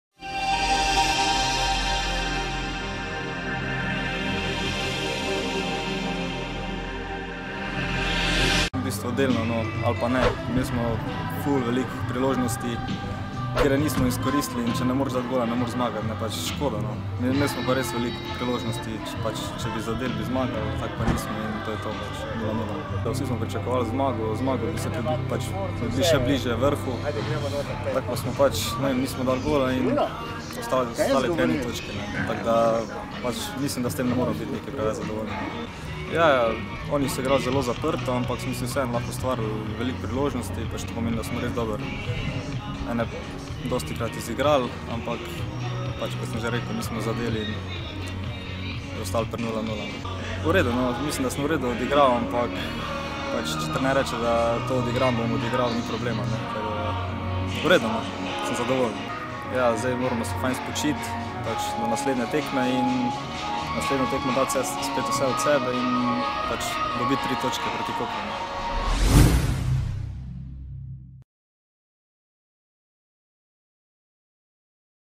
Izjave Baskere in priložnosti na tekmi Krka : Olimpija // NK Olimpija Ljubljana TV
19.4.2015
Krka : Olimpija 0:0 (0:0)

Zmaji so sicer prvič po povratku v prvo ligo osvojili točko na novomeškem Portovalu, a z njo nikakor ne morejo biti zadovoljni, saj so številne priložnosti ostale neizkoriščene.
»Krka je igrala zelo zaprto, a smo kljub temu uspeli ustvariti veliko priložnosti, ki jih žal nismo vnovčili. Z osvojeno točko ne moremo biti zadovoljni. Zdaj se moramo spočiti in proti Kopru doseči zmago,« je po tekmi povedal Rok Baskera, ki je tokrat zaigral na desnem bočnem položaju.
Več v video posnetku.

OTV
NK Olimpija Ljubljana TV